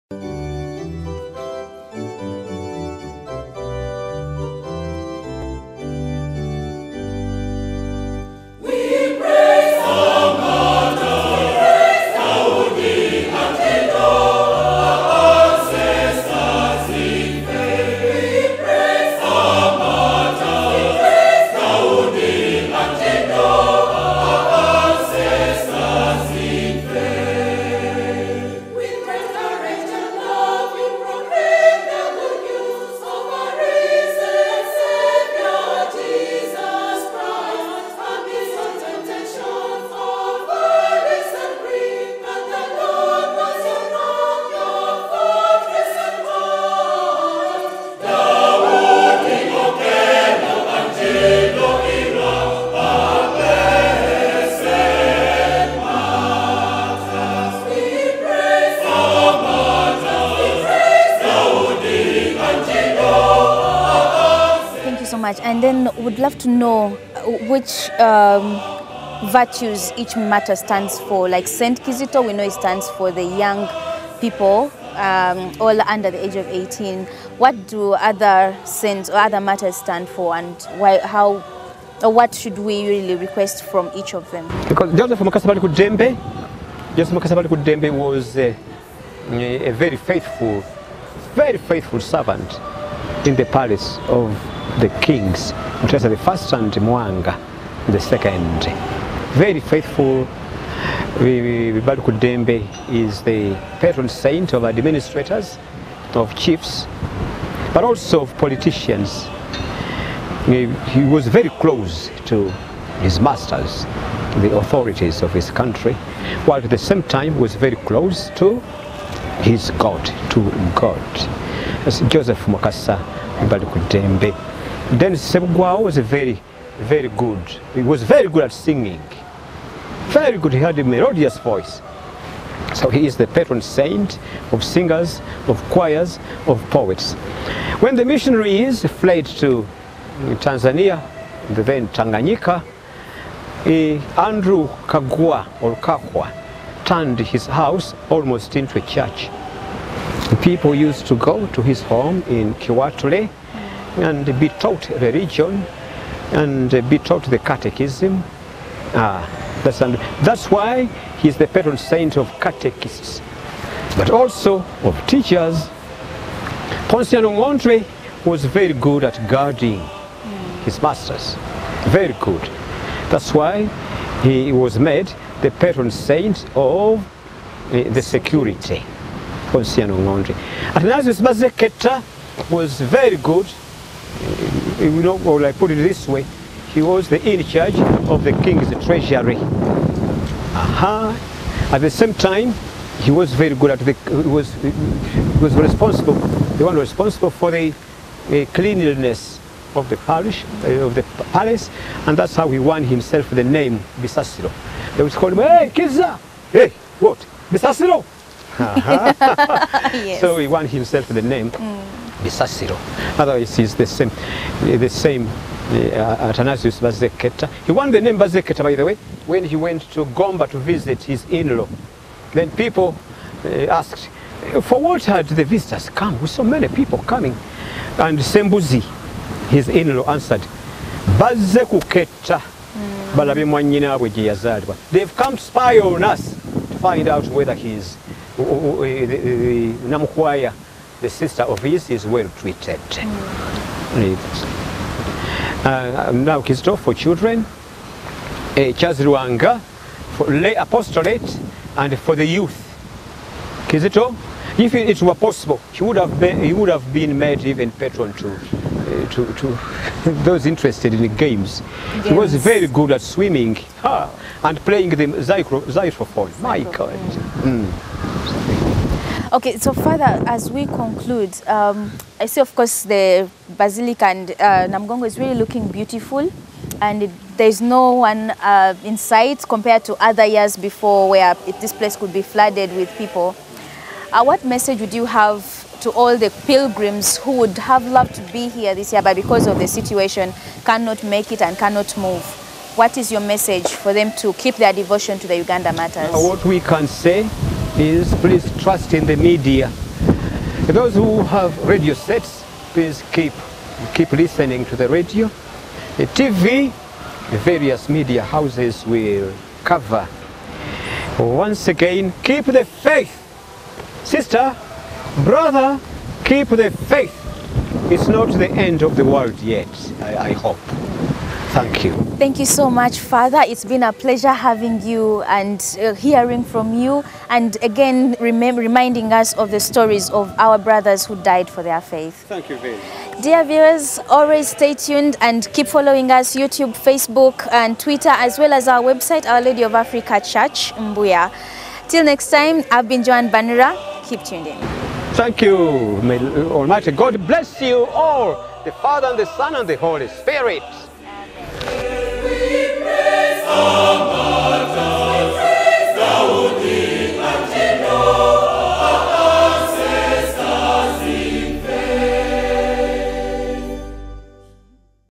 Virtues of the Uganda Martyrs by Fr Muwonge
Know the virtues of the Uganda Martyrs in our Interview with Rev. Fr. Joseph Mukasa Muwonge.